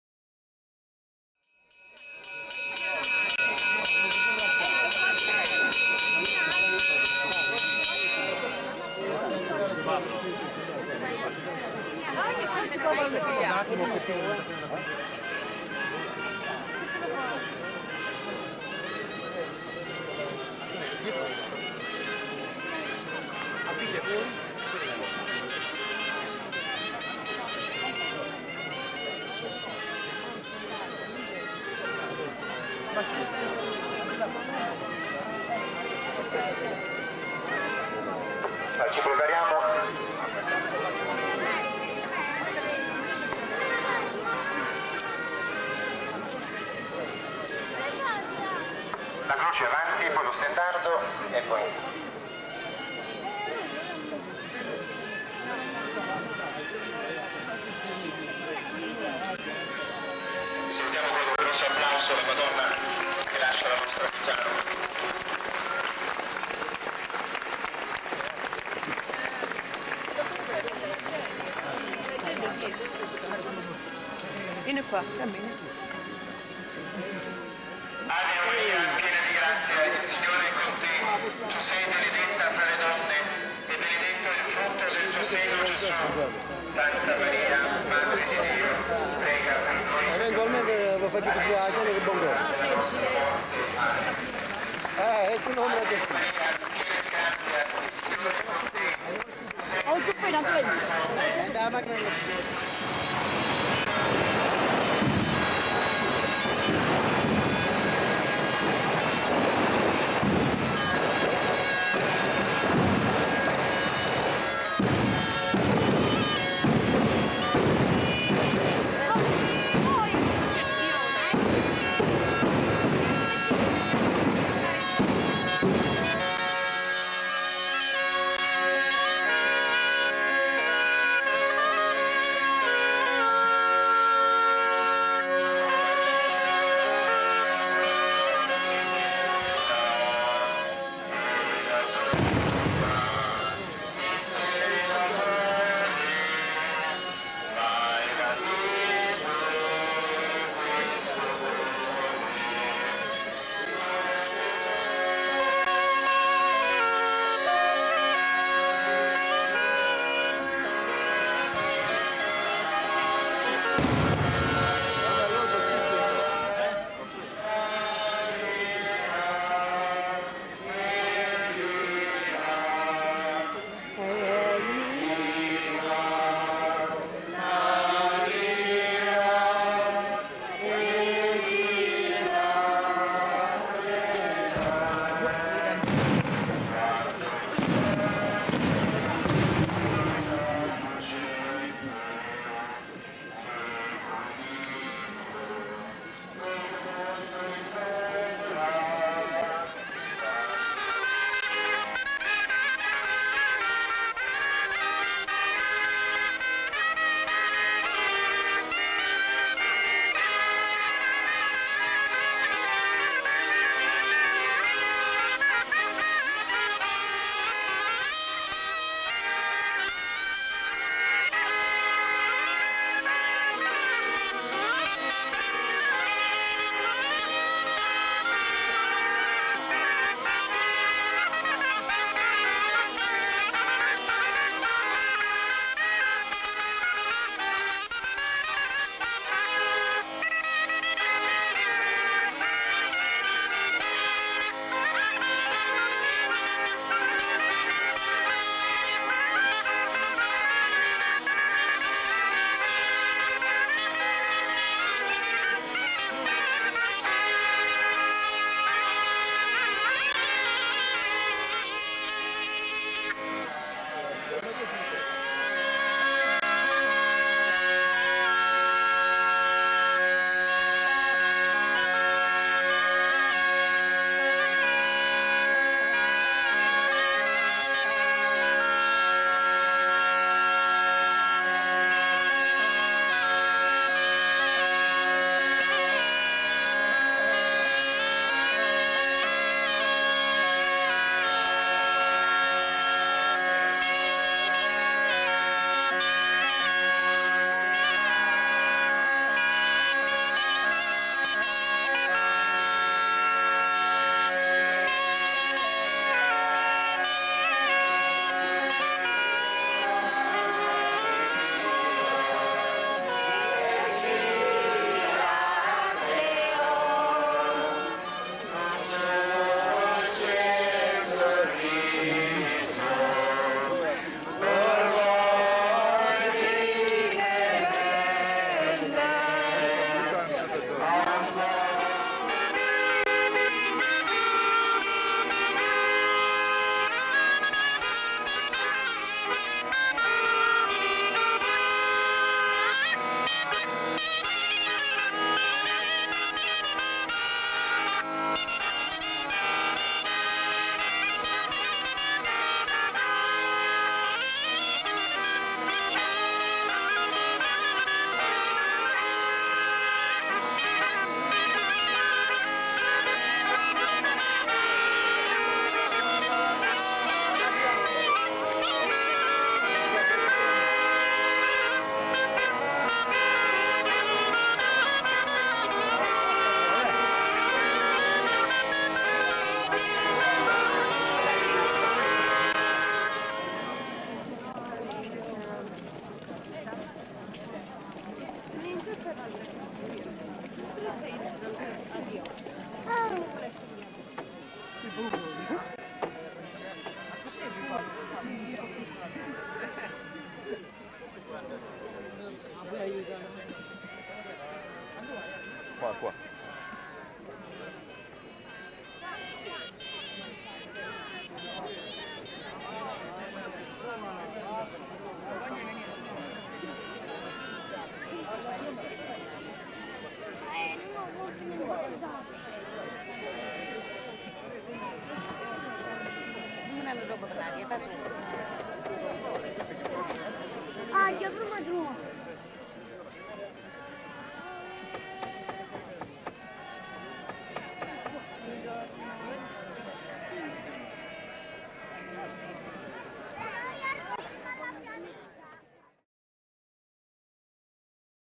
Viggiano maggio 1996 - La Partenza per il Sacro Monte
Ogni anno la prima Domenica di Maggio la Statua della Madonna viene accompagnata da Viggiano al Sacro Monte, dove resterà fino alla prima Domenica di Settembre.
Video di Amelio Taddeo per Viggiano Festa di gente Lucana e non solo